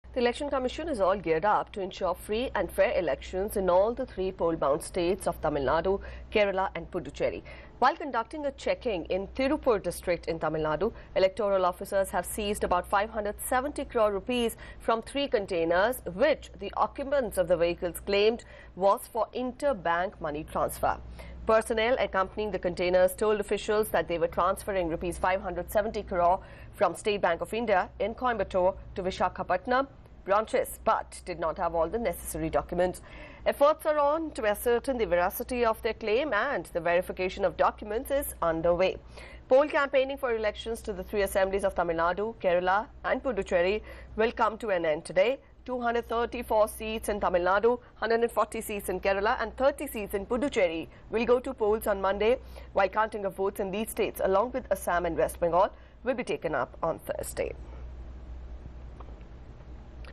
Poll campaign in Kerala, TN, Puducherry ends
Election commission is geared up to ensure free and fair elections in all three poll bound states of Tamil Nadu, Kerela and Puducherry. While conducting a checking in Tirupur district in Tamil Nadu, Electoral officials have seized about Rs 570 crore from three containers, which the occupants of the vehicles claimed was for inter-bank money transfer.